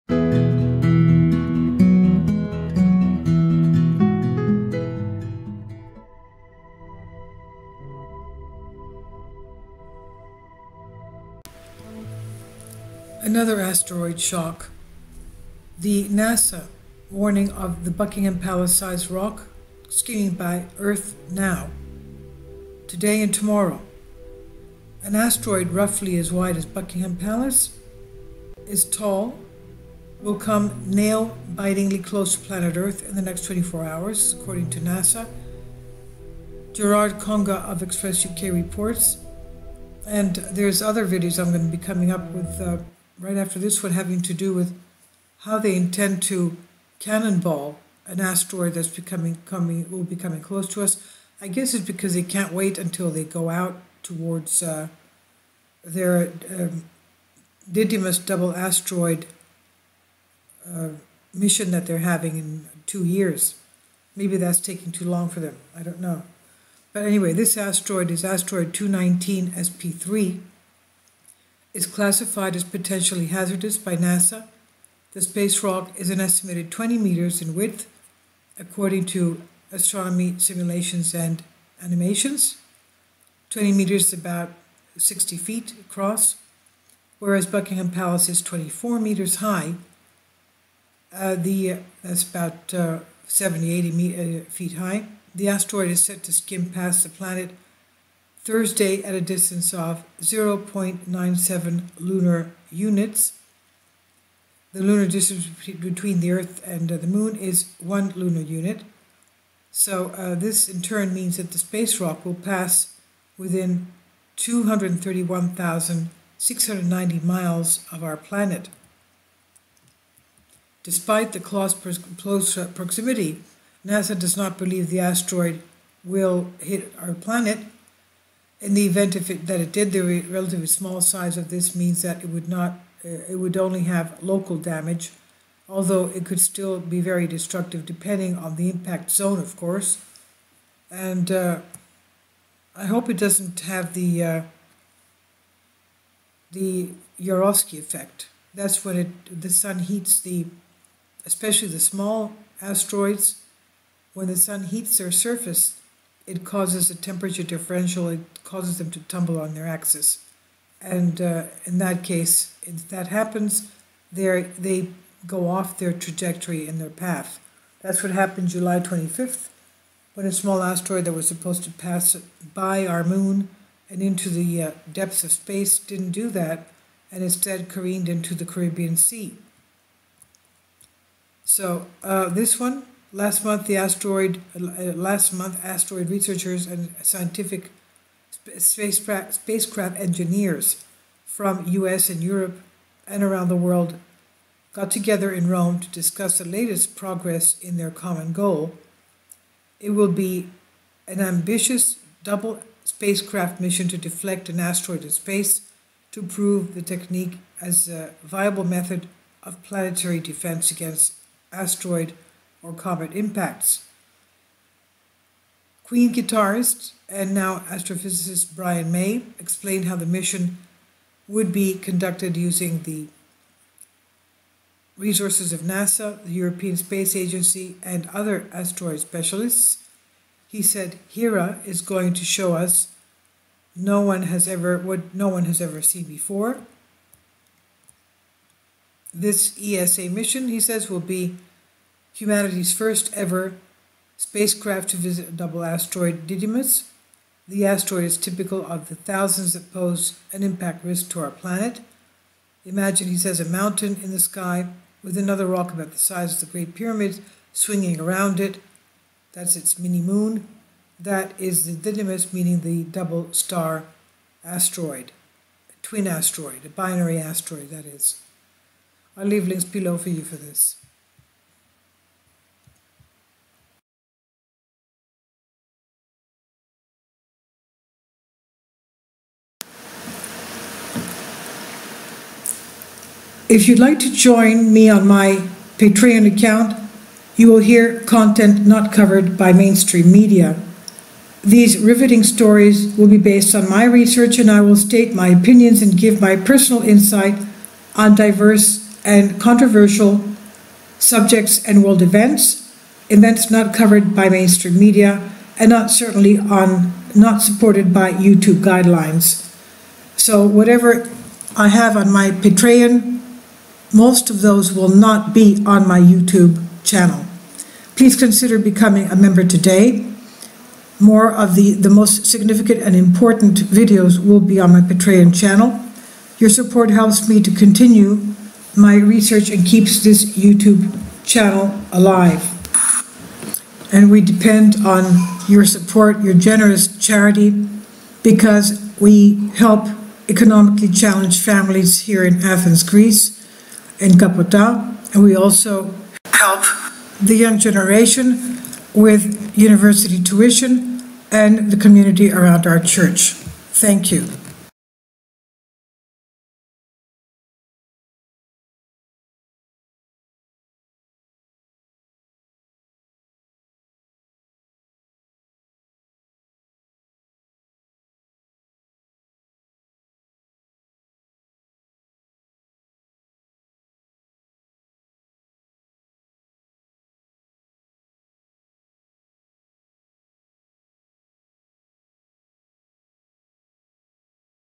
Asteroid Shock! NASA warn of Buckingham Palace-sized Rock to Skim by Earth in 24 hours!
2) Thumbnail image - getty/astronomy simulations and animations